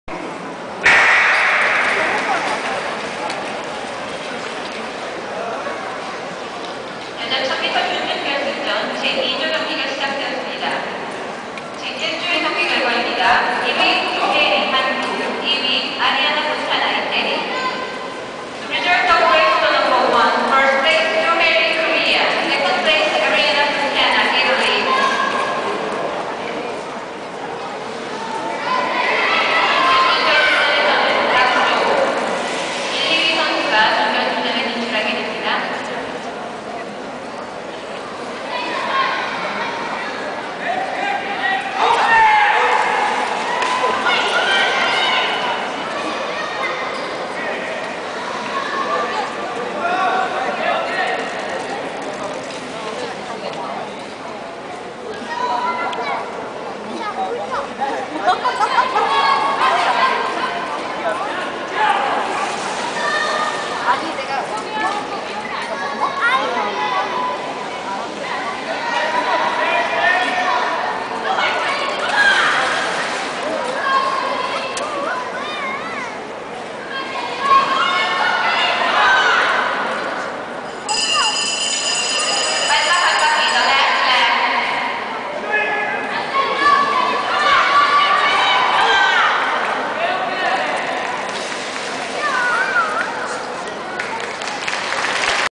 Short Track World Cup.Seoul.1000m.Ladies.Quarter.f.(2).2009.
Heat 2
PlaceStart Pos.NameISU MemberResultsQual.
1 2 ZHOU Yang CHN 1:33.286 Q
2 3 REUTTER Katherine USA 1:33.411 Q
3 1 ROBERGE Kalyna CAN 1:33.718
4 4 BORODULINA Tatiana